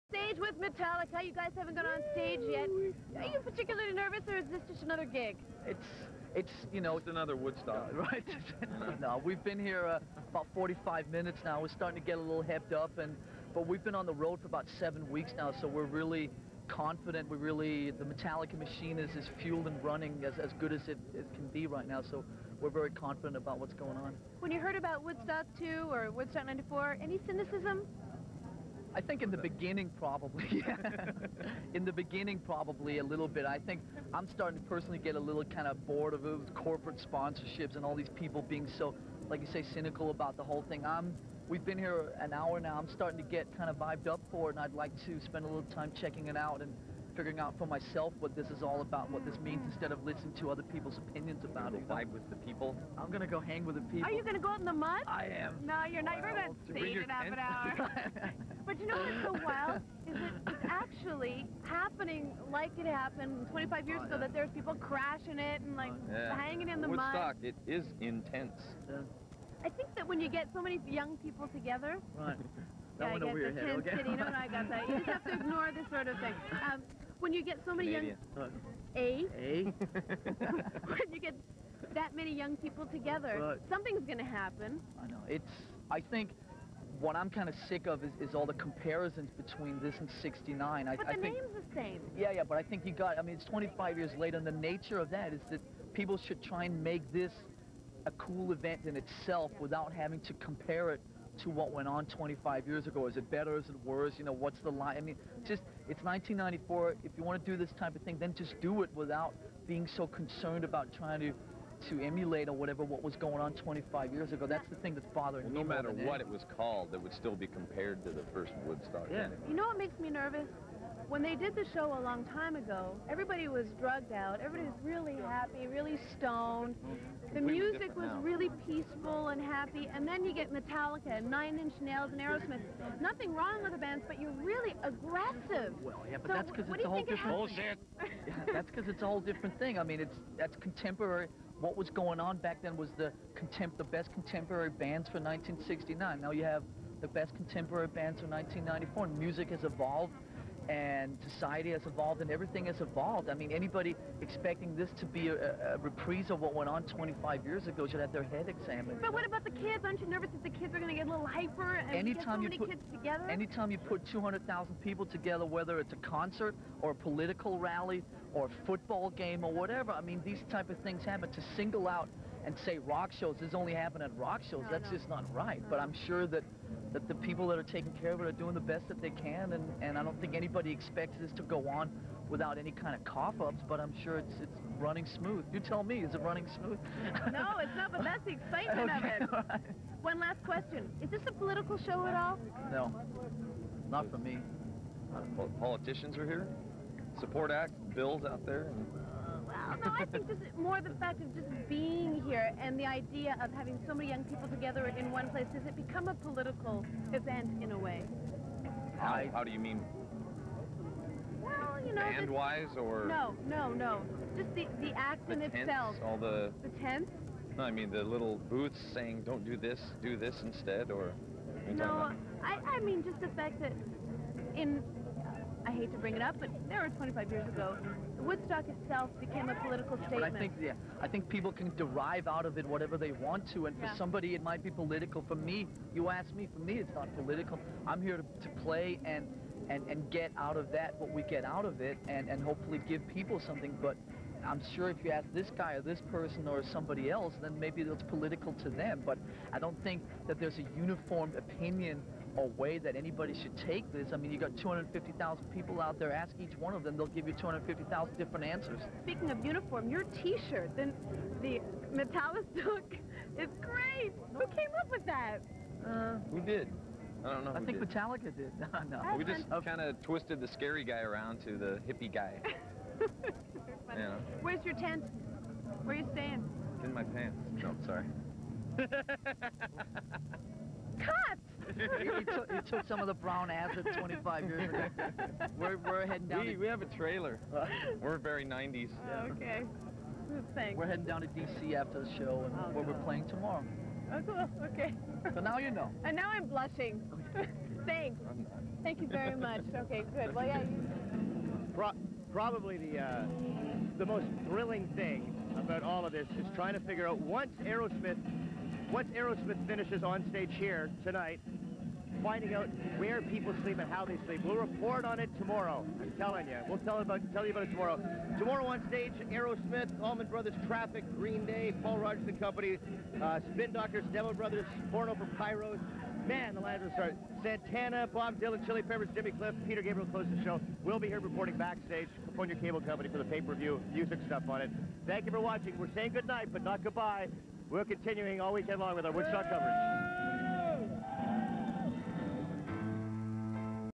Erica Ehm Interviews Metallica @ Woodstock '94
Erica Ehm Interviews Lars and James from Metallica @ Woodstock '94
Much Music
Steve Anthony talks about accommodations at Woodstock.